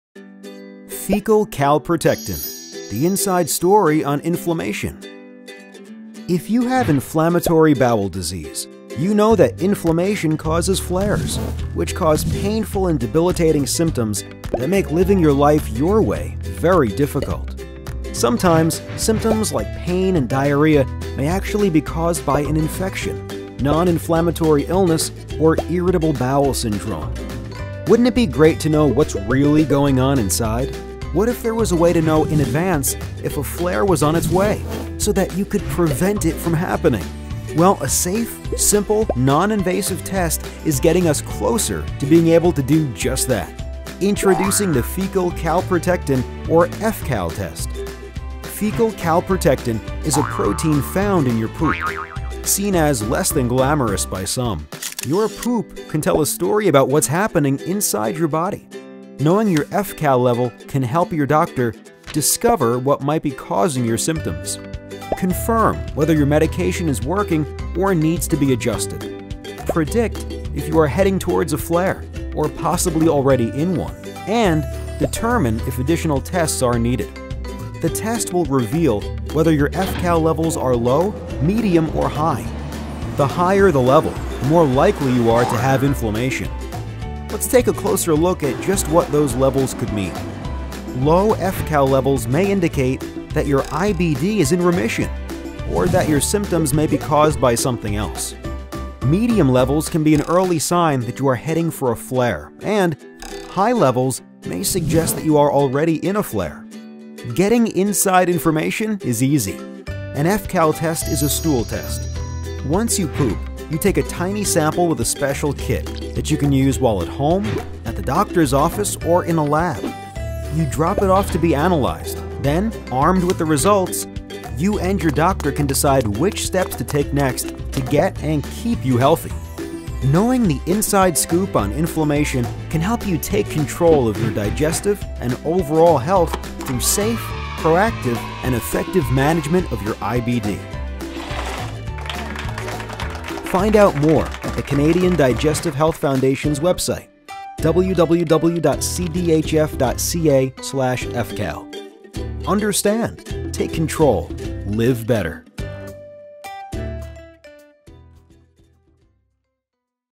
Fecal Calprotectin: The Inside Story on Inflammation
​If you have IBD, you know the painful symptoms you suffer from during a flare are caused by inflammation.  Wouldn’t it be great to know -- in advance -- if a flare was on its way, so that you could prevent it from happening!  Well, a safe, simple, non-invasive test could help you do just that! Watch the CDHF's latest animation on the Fecal CalProtectin -- or fCal -- test now.​